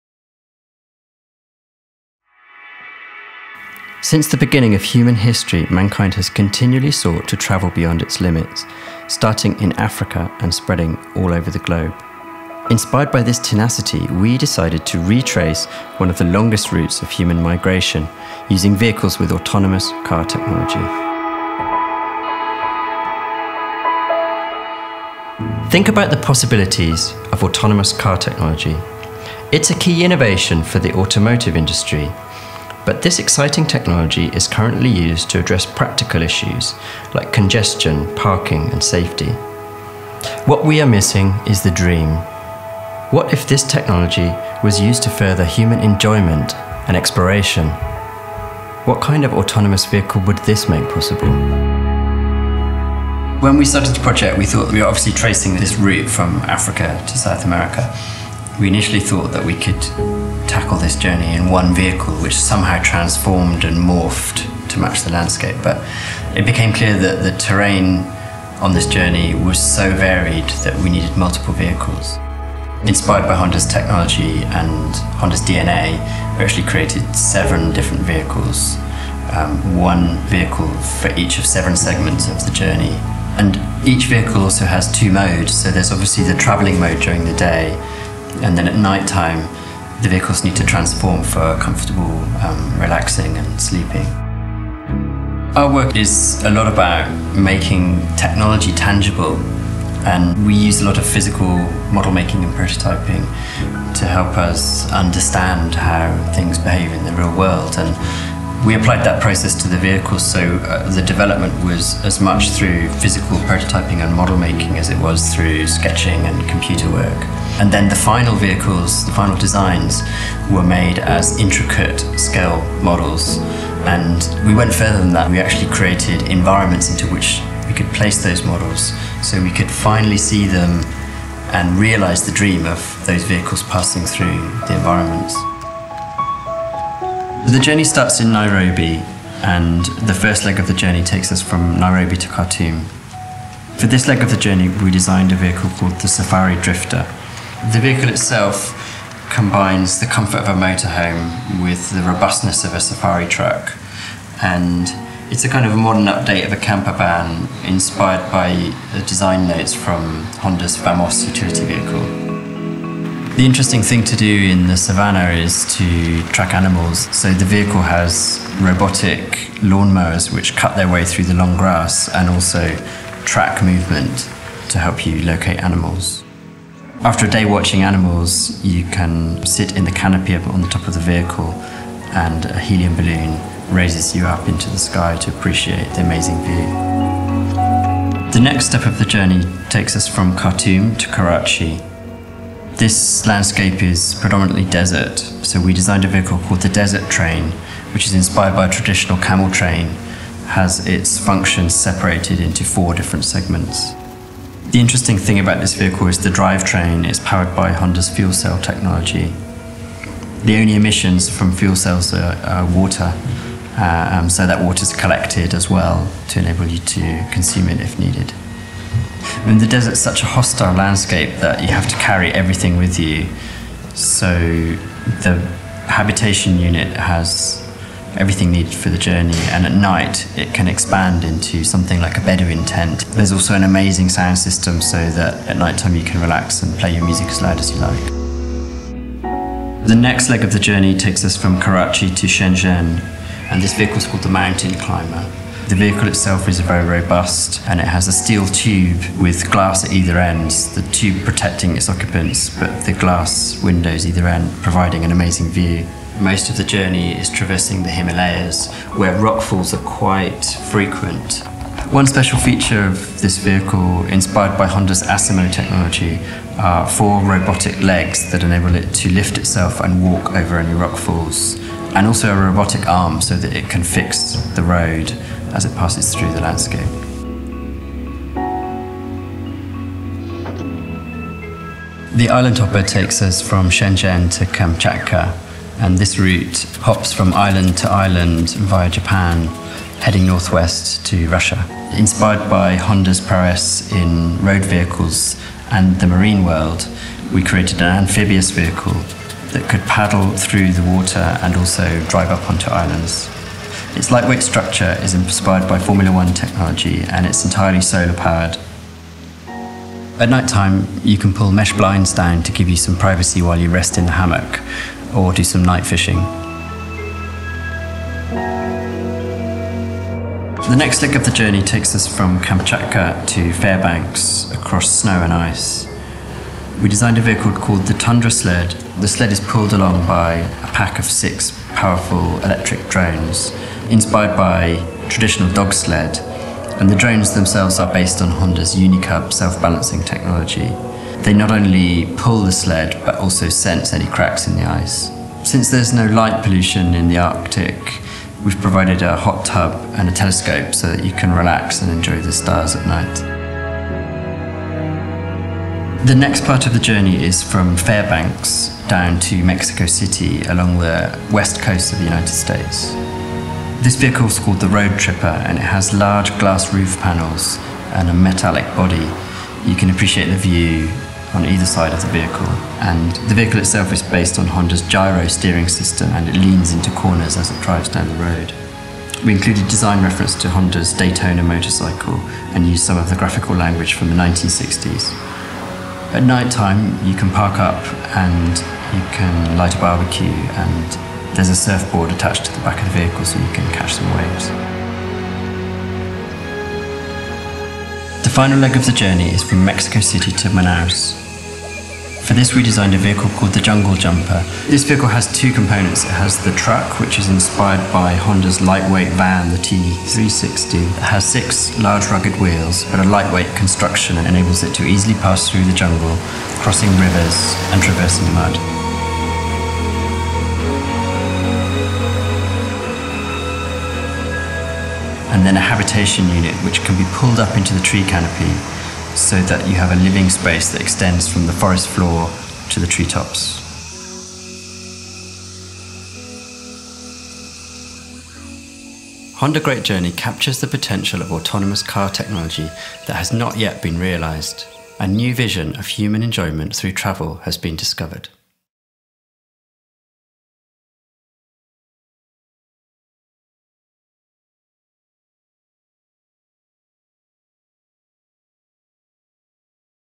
Honda. Great Journey.
‘Honda. Great Journey.’ showcases how autonomous driving could take us across the world. Map Project Office and Mori Inc. were commissioned by Honda to create seven concept vehicles suitable for a journey from Africa to Latin America, travelling over 36,000 km from Nairobi, Kenya to Manaus, Brazil through desert, mountains, snow and water. This project presents Honda’s future vision for autonomous driving technology in a context which has not previously been explored: Until now, discussions about autonomous driving have referred to safety or efficiency, but Honda wanted to show how this exciting new technology can enhance 'the joy of mobility’.